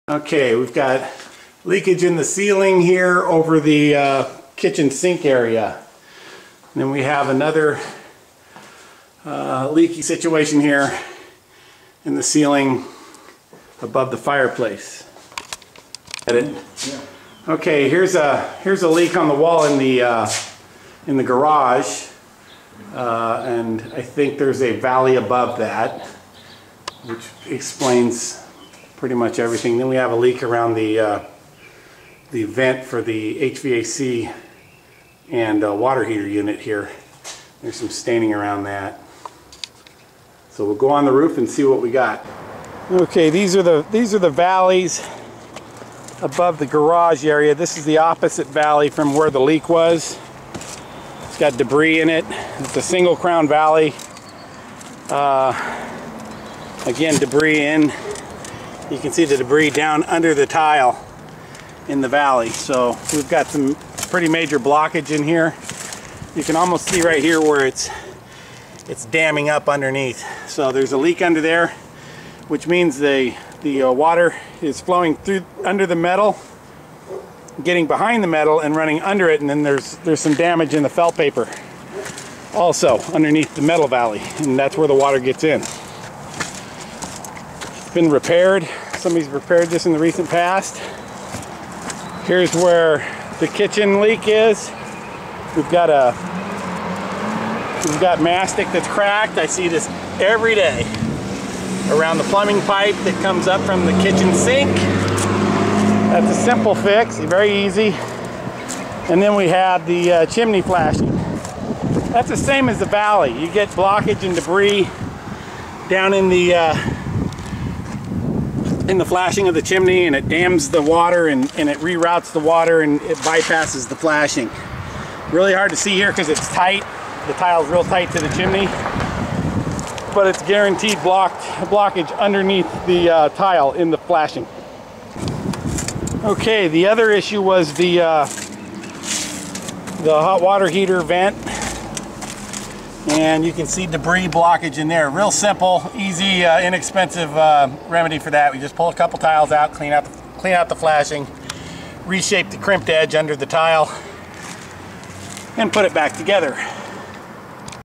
Roof Doctors | Roof Repairs Inspections Richmond California
Adam Wilfley | Roof Doctors | describes leaks inside of a home in the kitchen, near the chimney, above the garage, and near a plumbing vent and the roof sources of those leaks on a home in Richmond California .   Please contact our office for a free roof inspection or a free roof estimate.